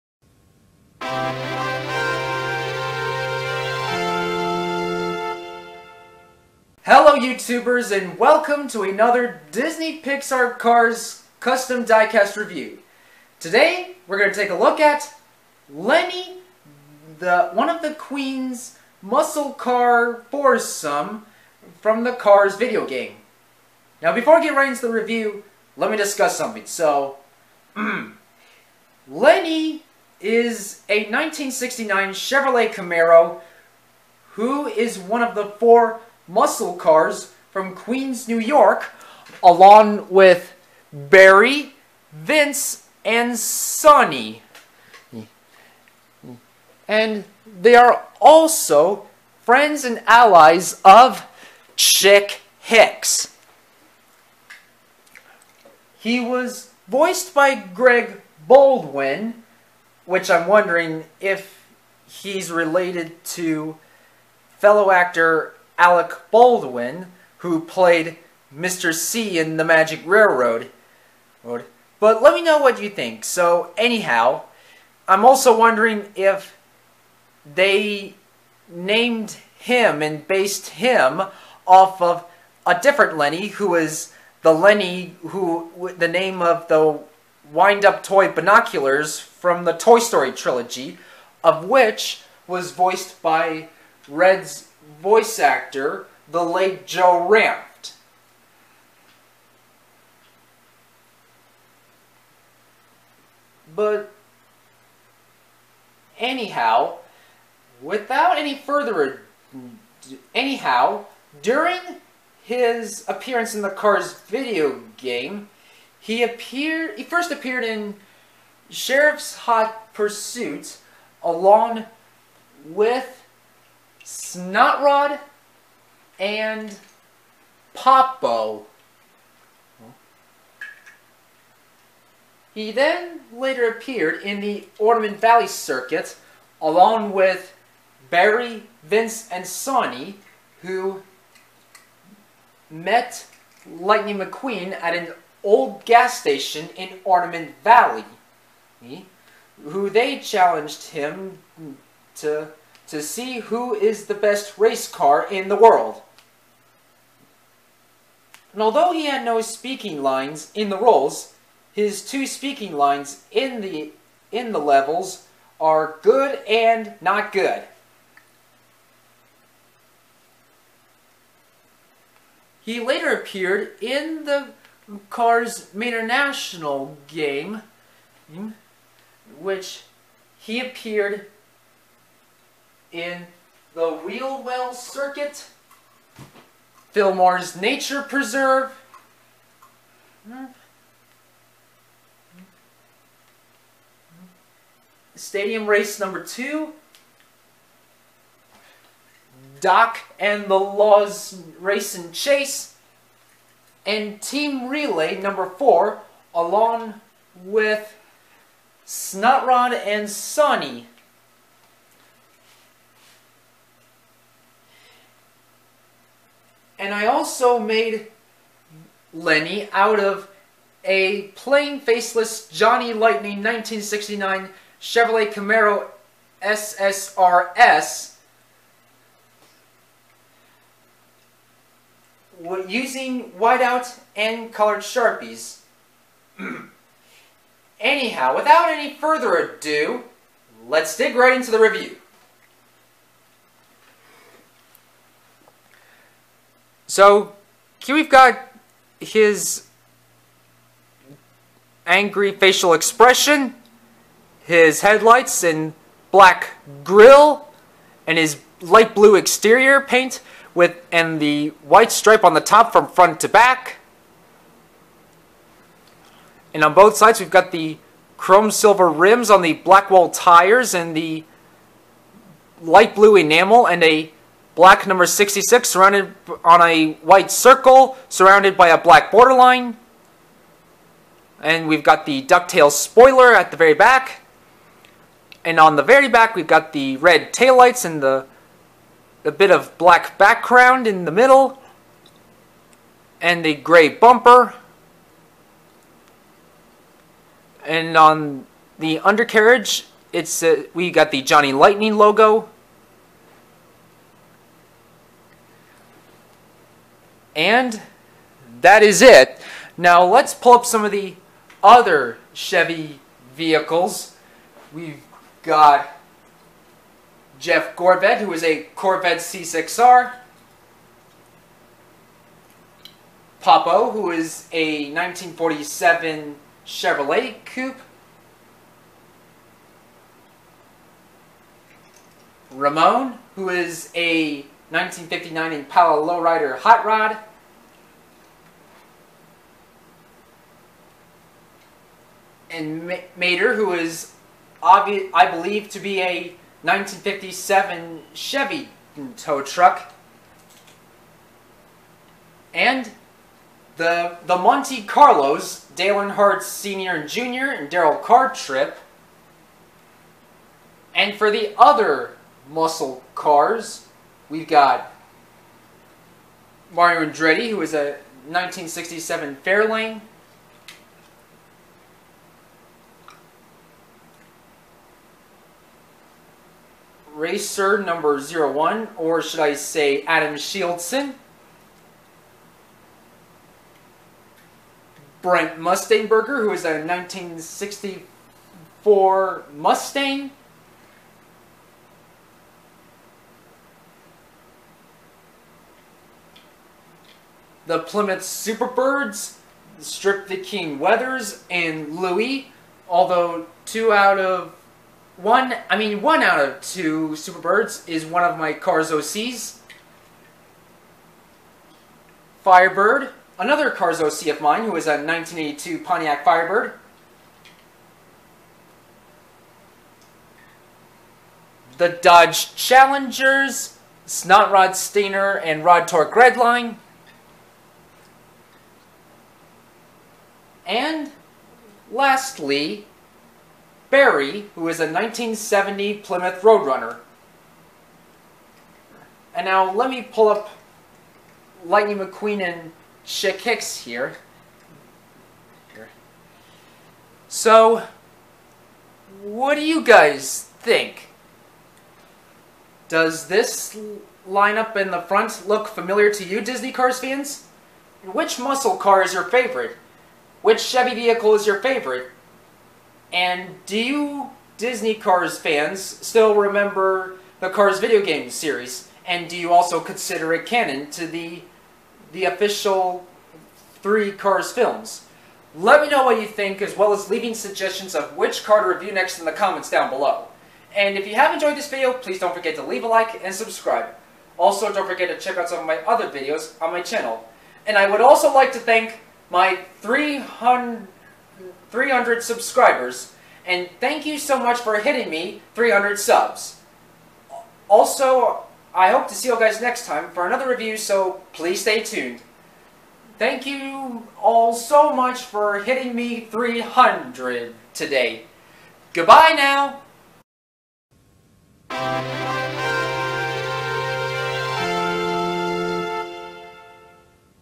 Disney Pixar Cars video game Custom Lenny (Queens muscle car) diecast
Hello everybody & today, we're gonna take a look at Lenny from Cars the video game. I would like thank everybody for hitting me 300 subscribers, today!
Cars belongs to Disney/Pixar.
1969 Camaro is a trademark of Chevrolet.